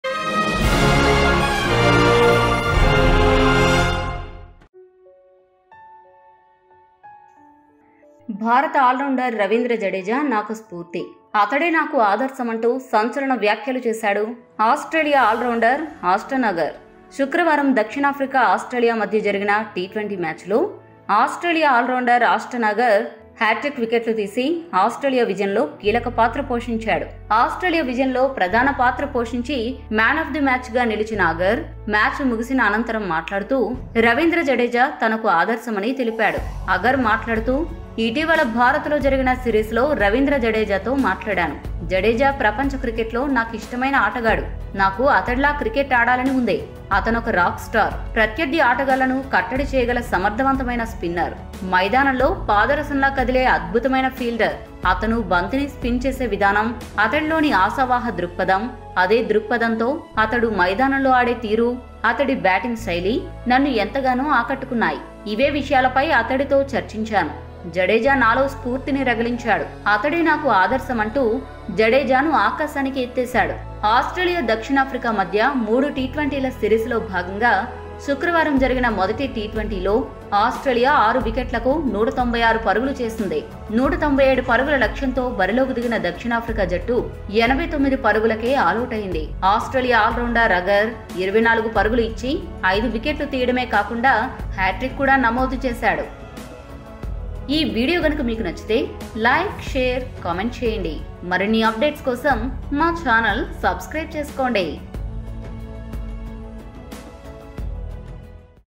Ashton Agar Superb Words About Ravindra Jadeja After Hat Trick Against South Africa|Cricket News
Ashton Agar Superb Words About Ravindra Jadeja After Hat Trick Against South Africa|Cricket News|Filmy Poster